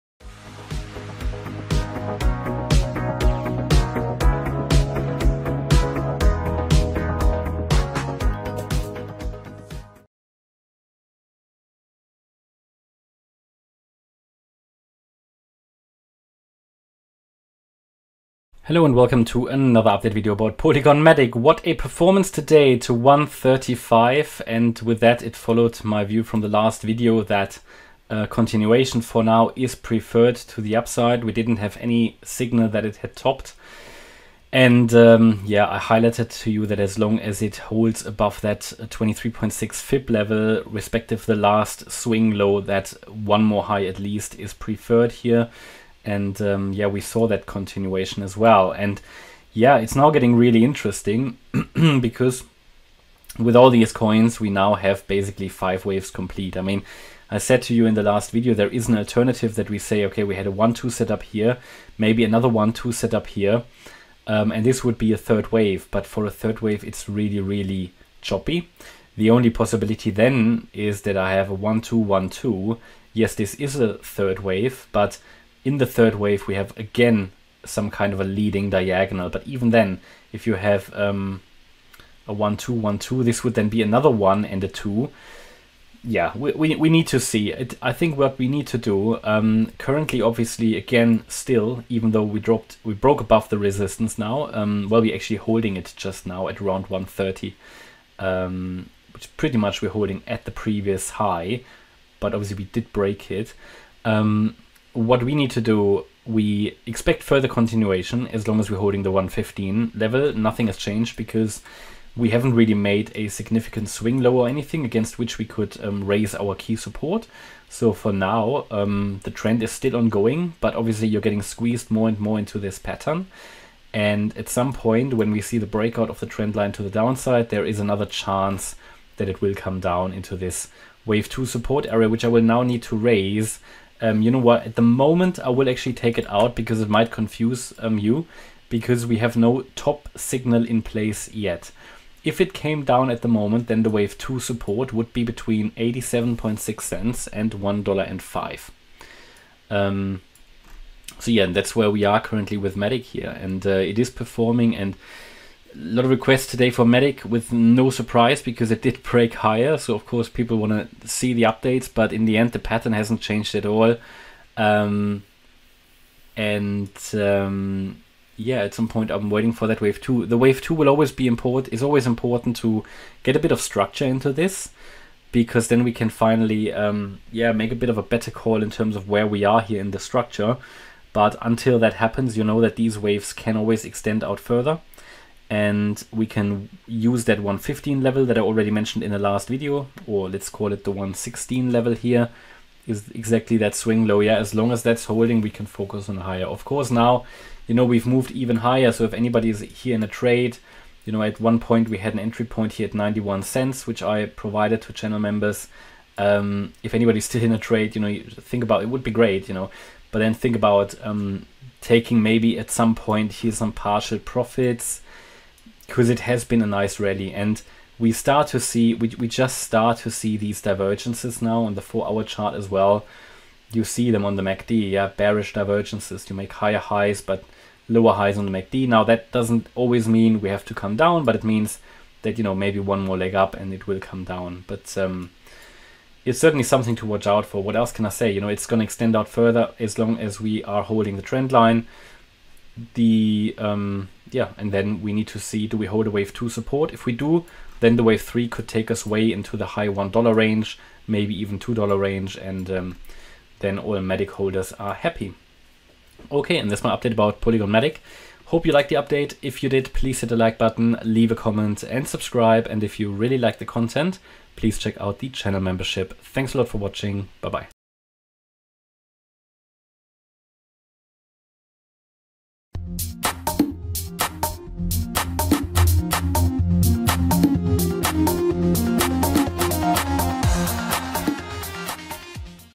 Polygon MATIC Price News Today - Elliott Wave Technical Analysis Update, This is Happening Now!
Polygon MATIC Price News Today - Elliott Wave Technical Analysis Update, This is Happening Now!  In this video I talk about a technical analysis of the Polygon MATIC price and I talk about some important price moves that are happening right now!  Polygon MATIC Price Prediction!

Trade on Coinbase:
Receive $10 in BTC (see terms on website):

Trade with Trading212:
Receive shares worth up to $100: